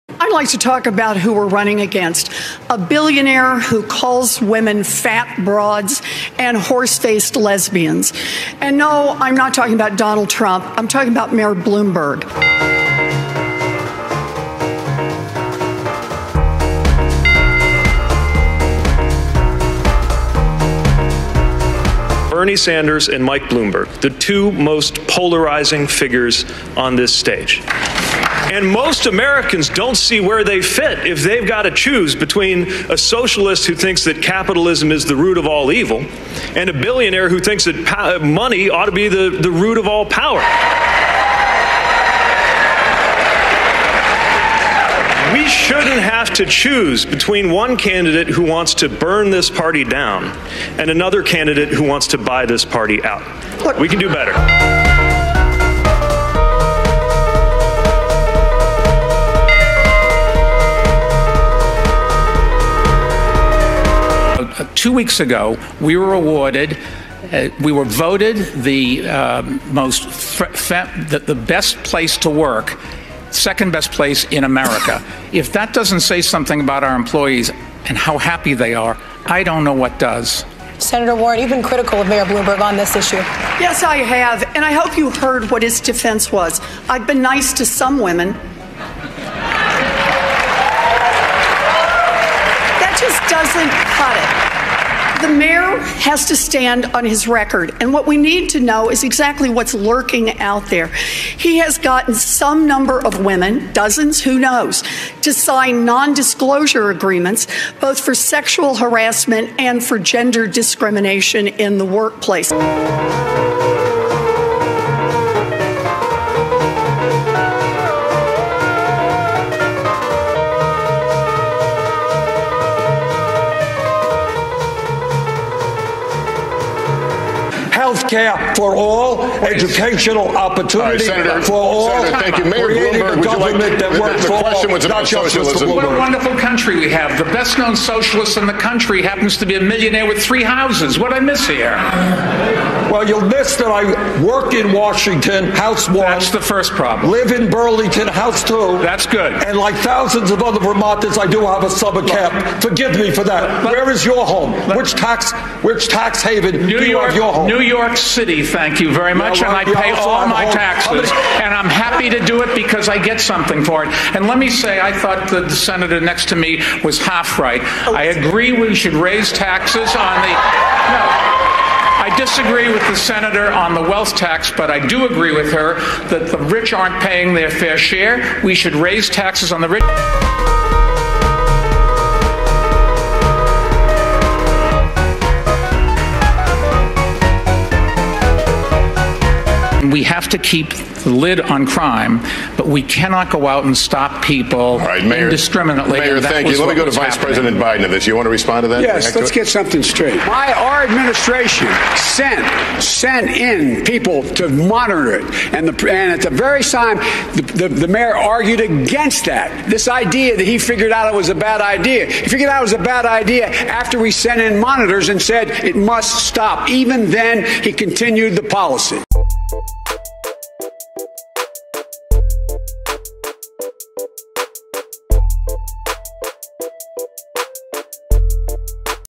Bloomberg Gets OWNED By Elizabeth Warren At Democrat Debate In Nevada
2020 Democratic presidential candidate Michael Bloomberg was attacked by Elizabeth Warren and all the other Democratic presidential candidates at the Democratic debate in Las Vegas, Nevada.